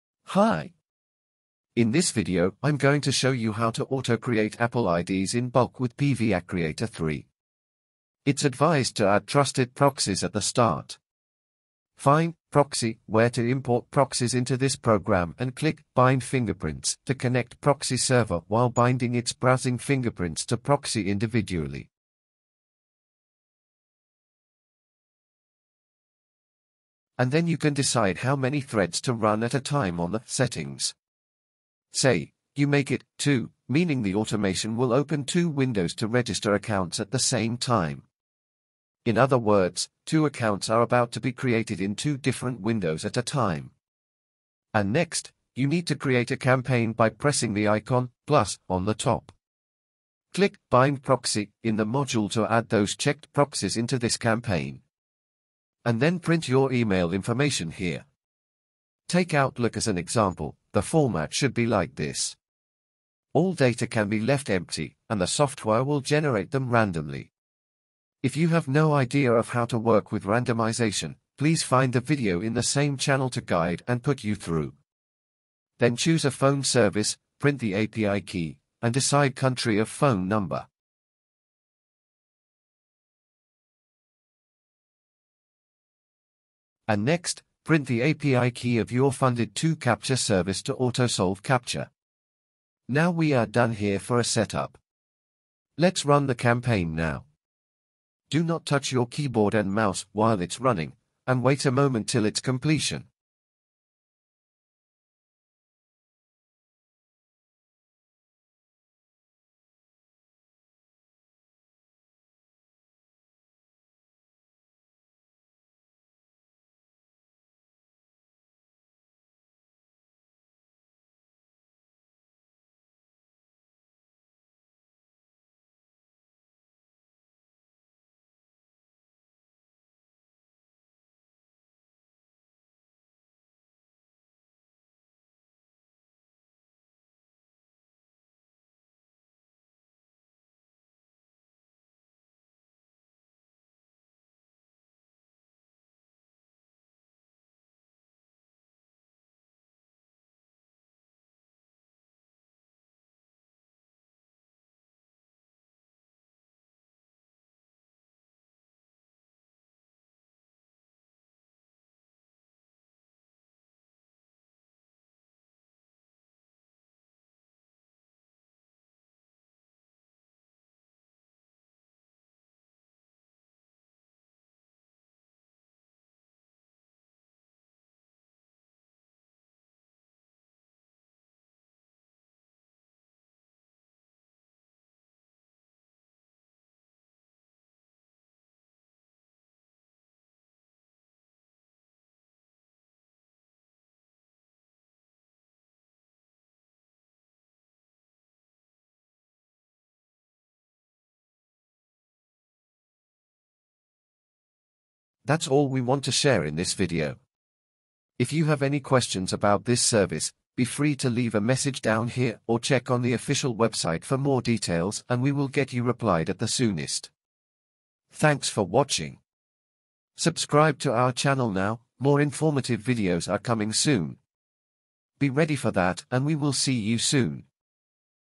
PVACreator Apple ID - Auto Create Apple Accounts in Bulk-How to Create Apple ID without phone number
liyan40off

PVACreator is an account creation bot. It can batch create accounts of more than 30 popular websites, such as Facebook, Gmail, Outlook, Twitter, SoundCloud. It can create high-quality phone verified accounts without the need for users to provide numerous phone numbers. It also solves the captcha automatically and allows you to import proxies to register your accounts.

create multiple Apple ID accounts

create unlimited Apple ID accounts